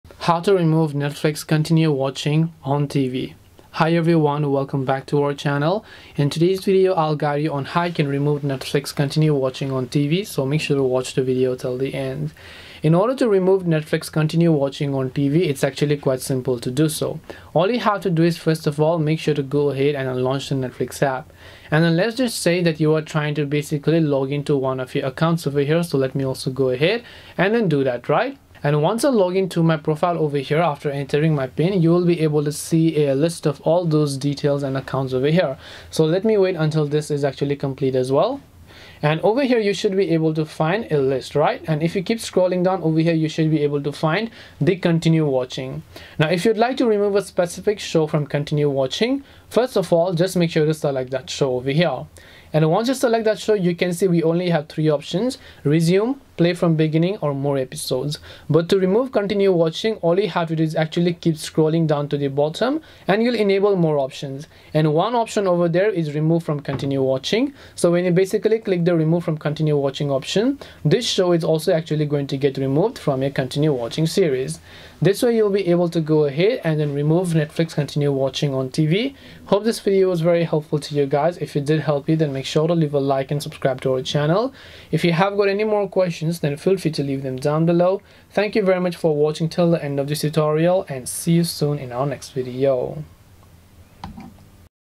How to Remove Netflix Continue Watching on TV !
➜ This video guides you through an easy step-by-step process to remove Netflix Continue watching on TV. So make sure to watch this video till the end.

~ Chapters:
0:00 Introduction
0:12 General Info
0:38 Remove Netflix Continue Watching
1:19 Conclusion